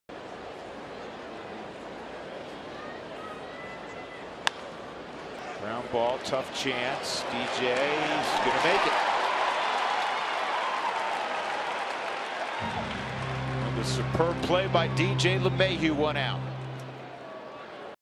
PHI@COL: LeMahieu makes sliding play, throws for out
DJ LeMahieu makes a nice sliding stop on a tough play and throws out Tyler Goeddel at first base